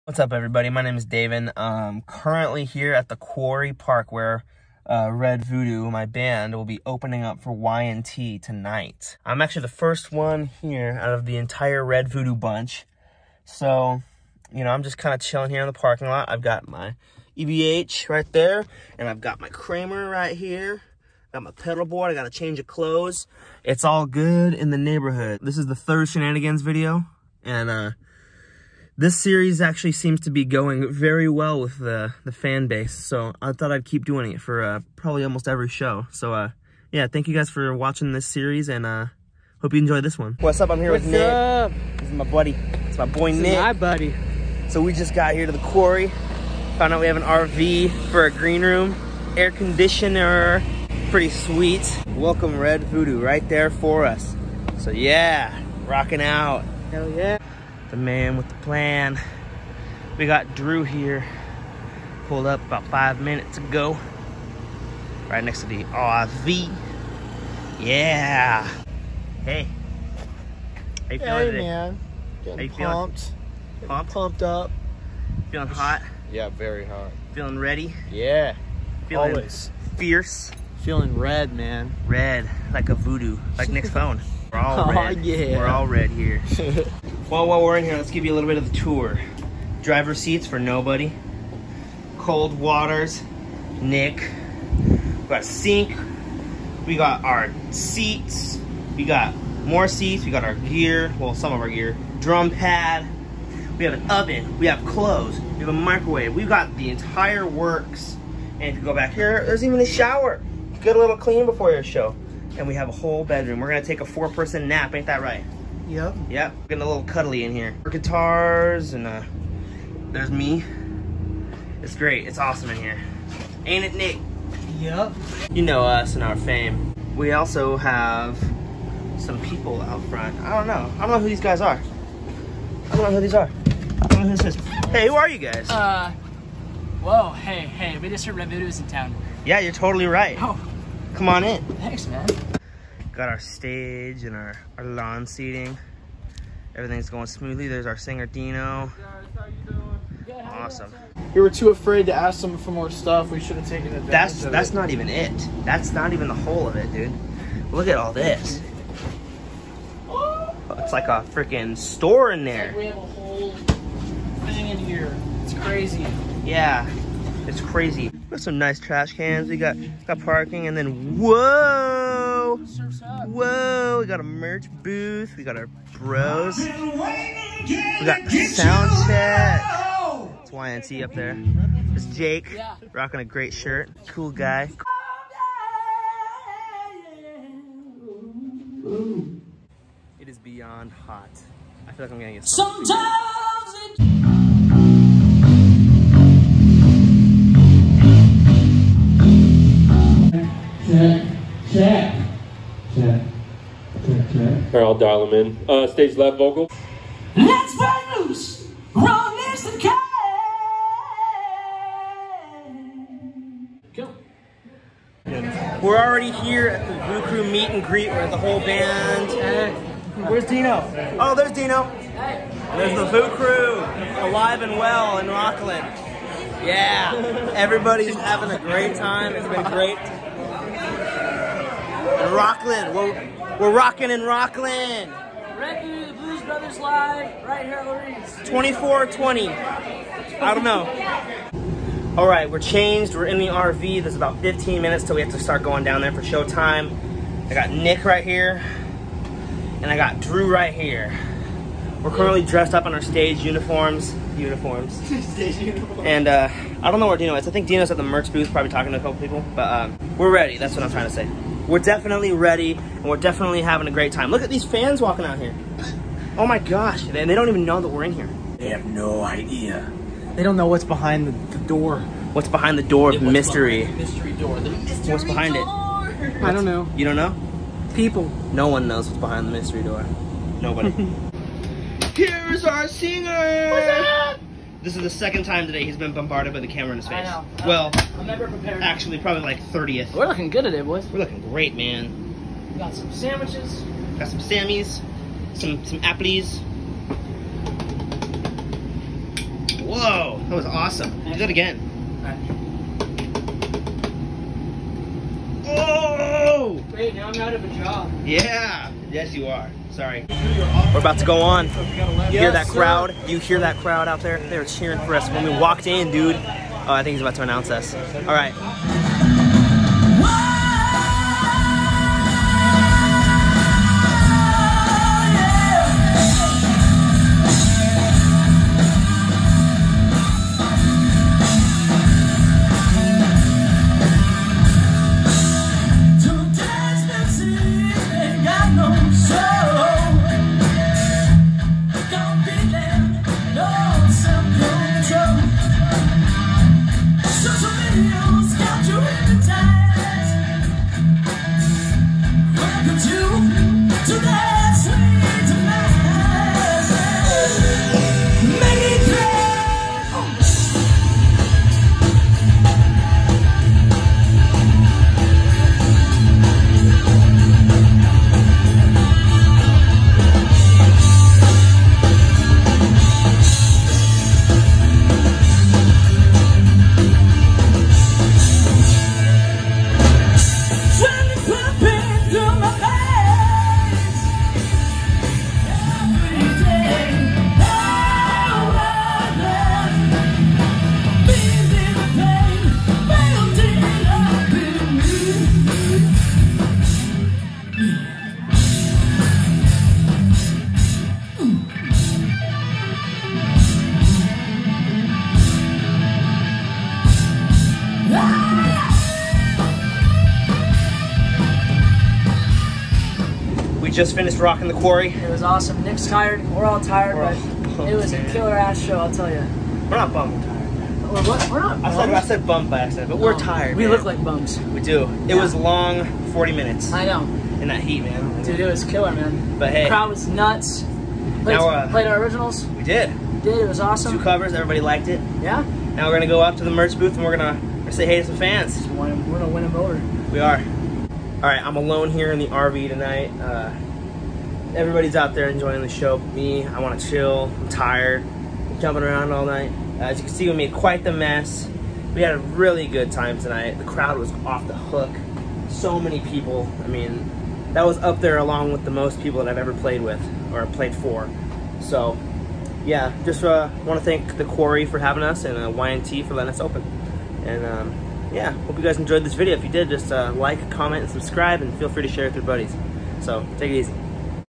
Shenanigans at the Quarry
Little behind the scenes video of our show with Bay Area legends, Y&T